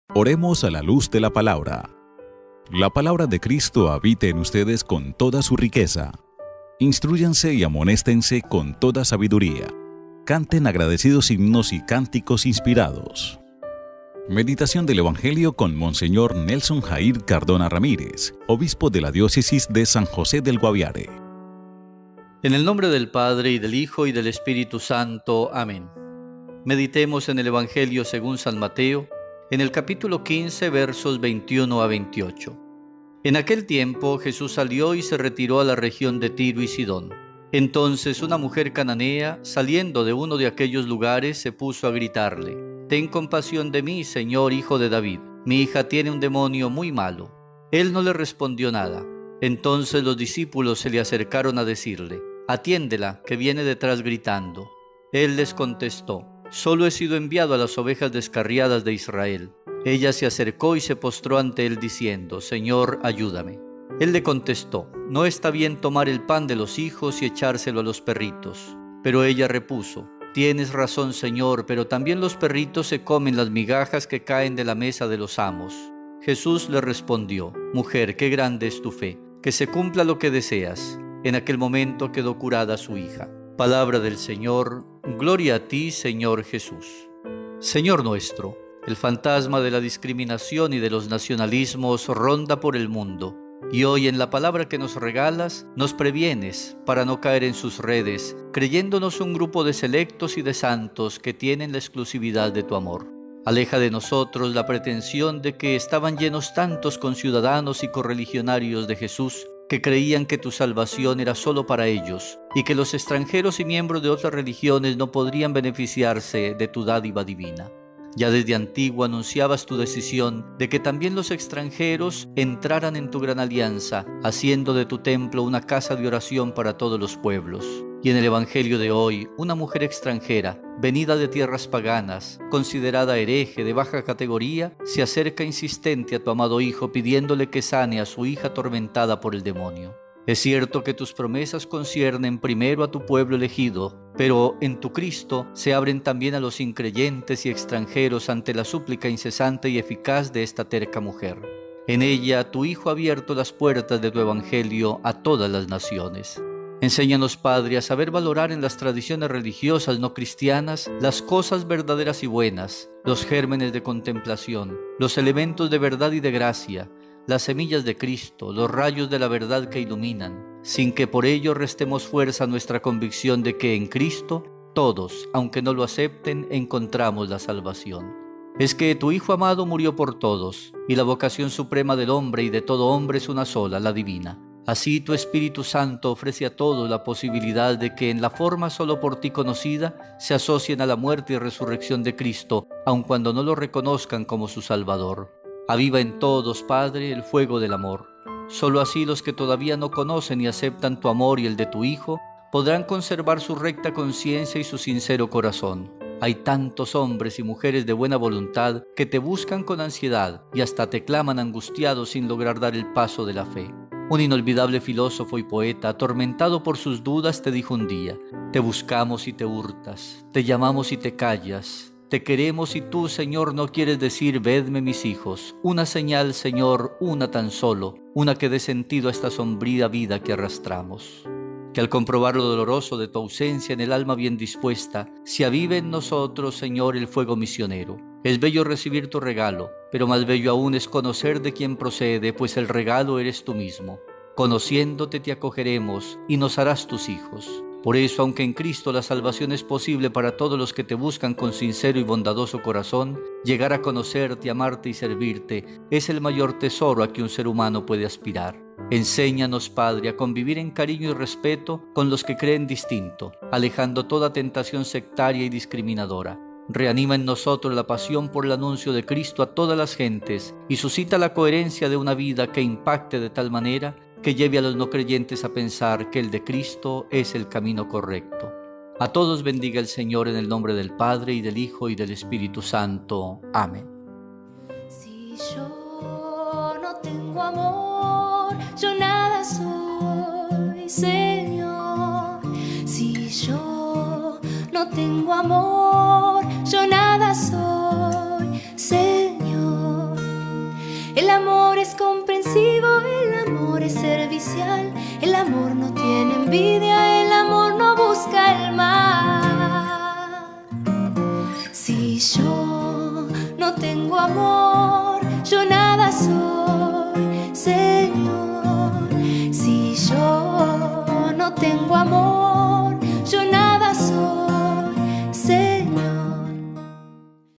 Reflexión Evangelio - Domingo XX del Tiempo Ordinario Ciclo A - Agosto 16 de 2020
Reflexión Evangelio - Domingo XX del Tiempo Ordinario Ciclo A - Agosto 16 de 2020 - Diócesis de San José del Guaviare - Monseñor Nelson Jair Cardona Ramírez.